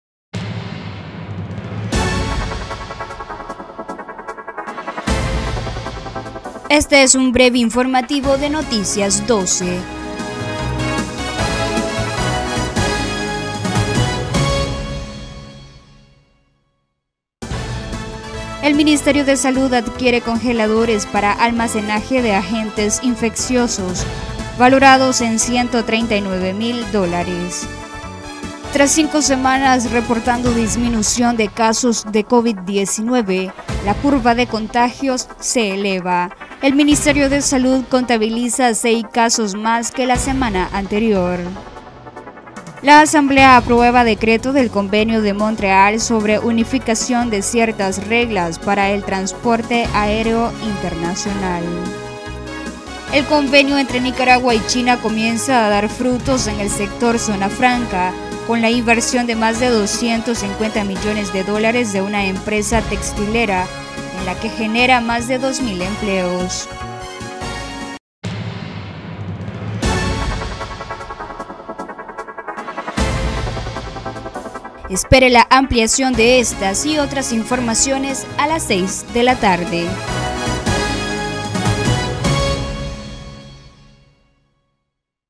Breve Informativo
📍Minsa adquiere congeladores para almacenaje de agentes infecciosos
📍Reporta aumento en los casos de covid-19
📍Asamblea aprueba decreto del Convenio de Montreal sobre Unificación de ciertas reglas para el transporte aéreo internacional
📍Nueva empresa textilera generará más de 2 mil empleos